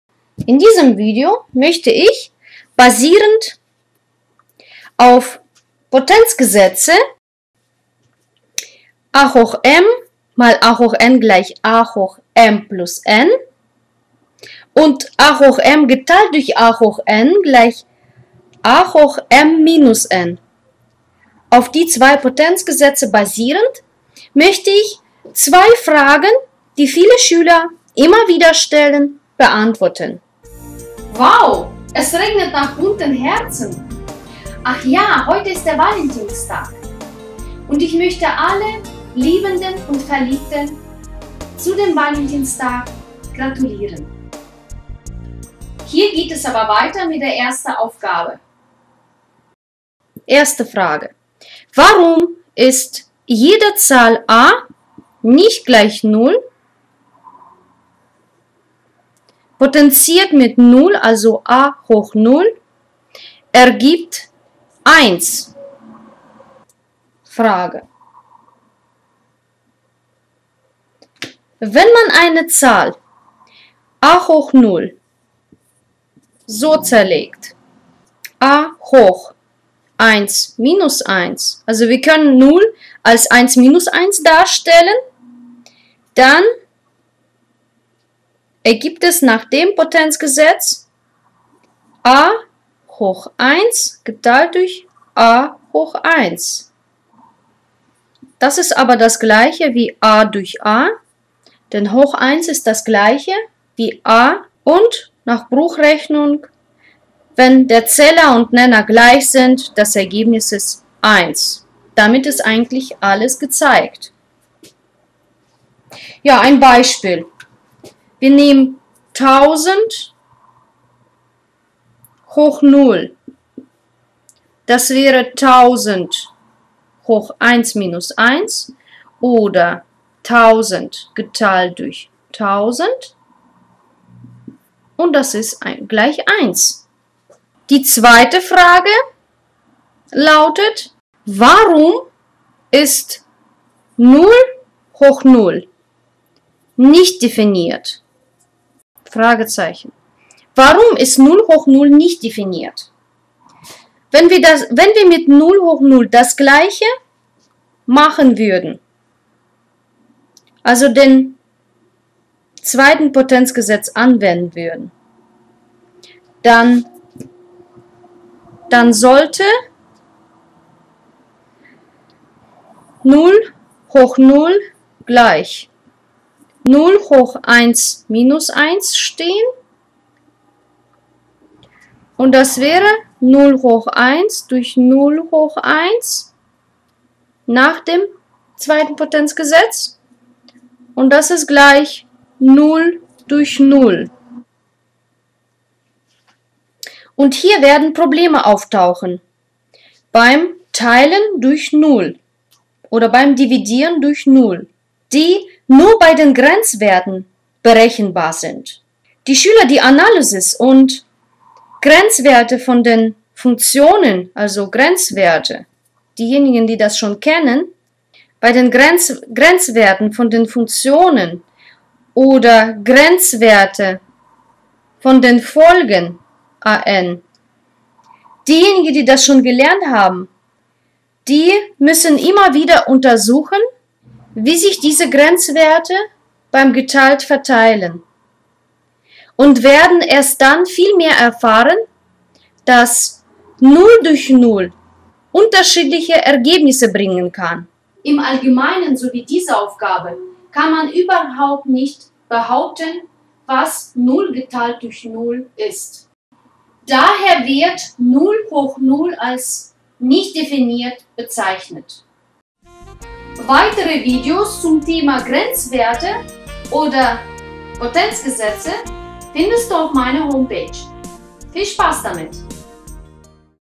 Warum ist Null hoch Null undefiniert und jede Zahl nicht gleich Null hoch Null eins ergibt
Null hoch Null undefiniert.
Jede Zahl nicht gleich Null hoch Null ergibt eins